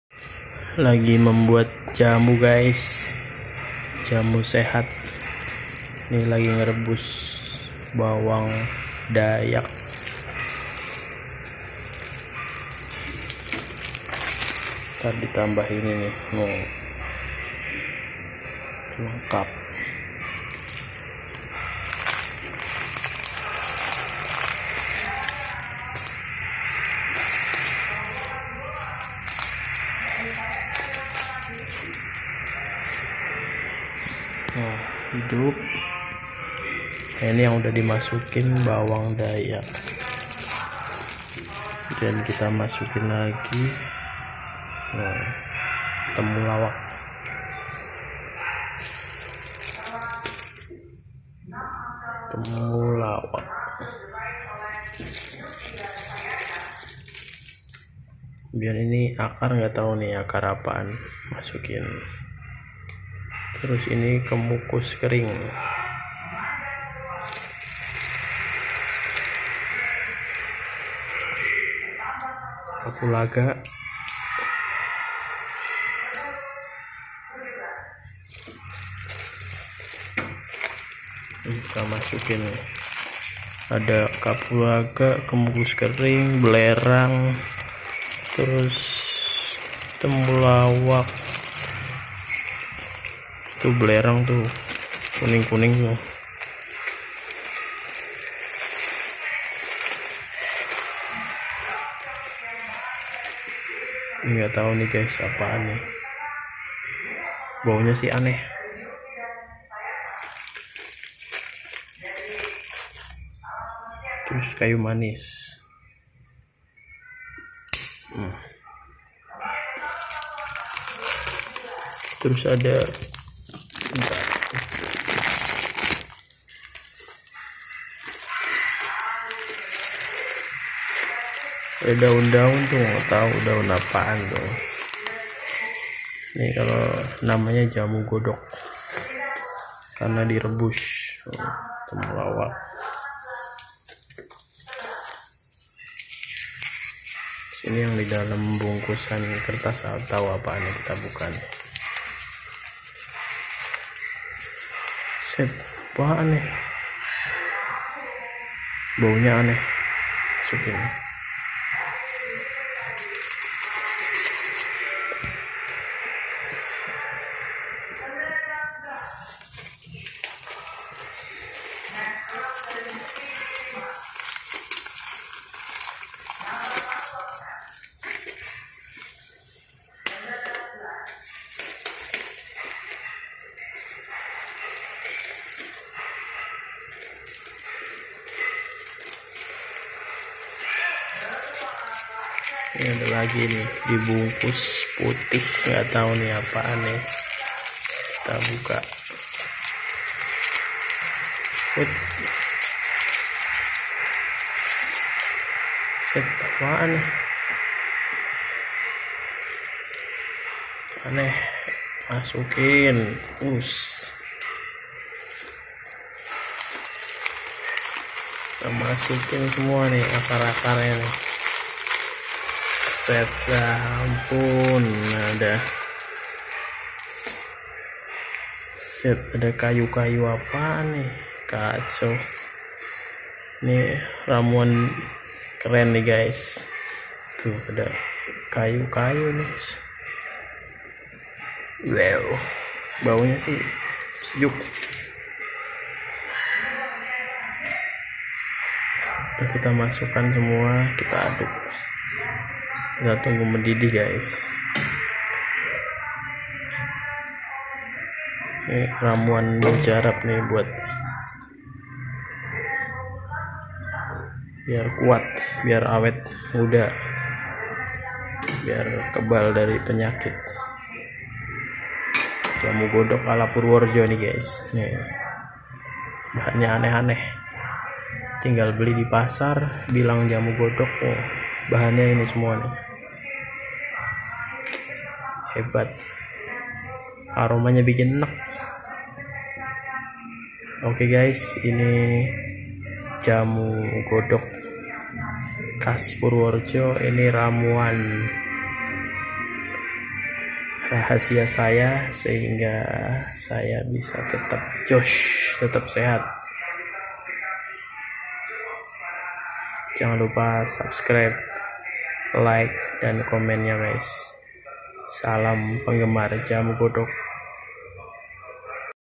Jamu obat kuat, ramuan ajaib dari Purworejo
Jamu Rebus khas Purworejo
untuk kekebalan tubuh
penambah stamina